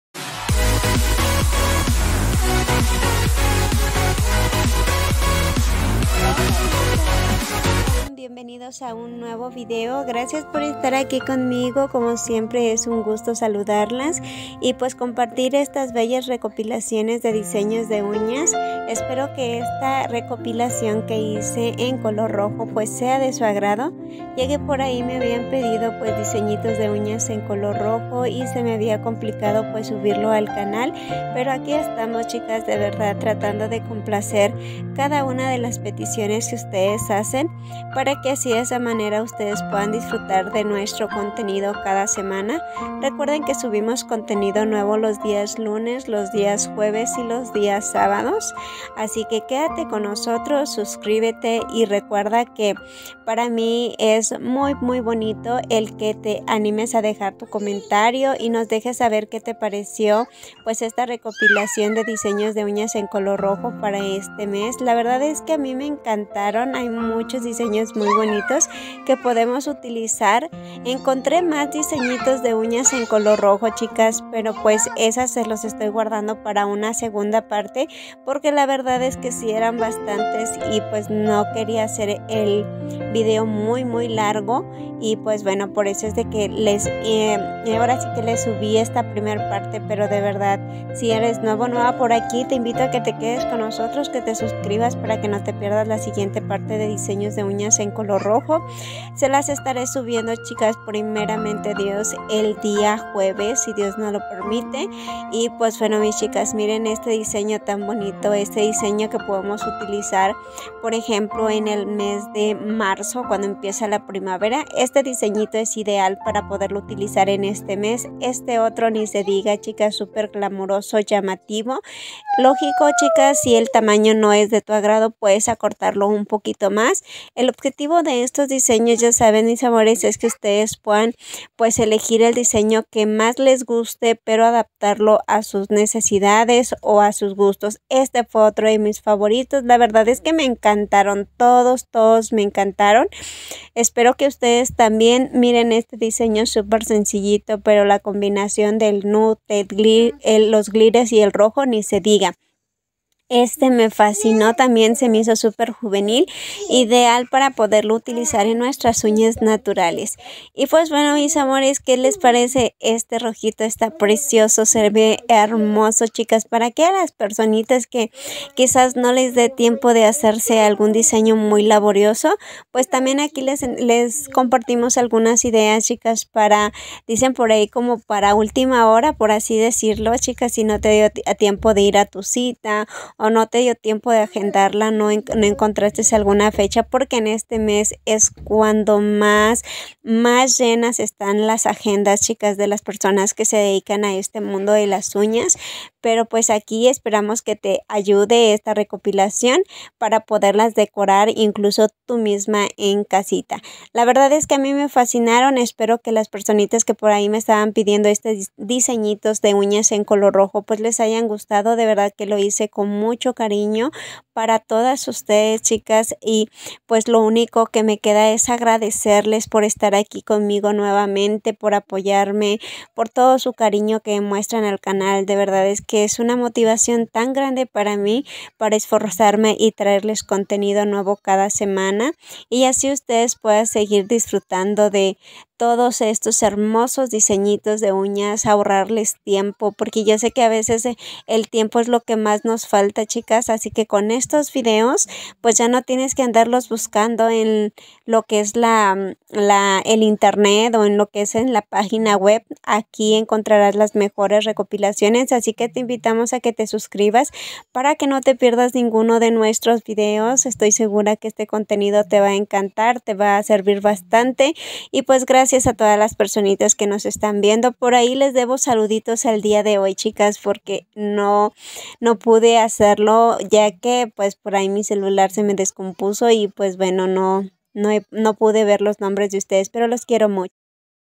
💅DISEÑOS DE UÑAS COLOR ROJO♥️uñasrojas2023
HOLA CHICAS BUENOS DÍAS HOY LES TRAIGO ESTOS HERMOSOS DISEÑOS DE UÑAS COLOR ROJO ESPERO LES GUSTE ♥️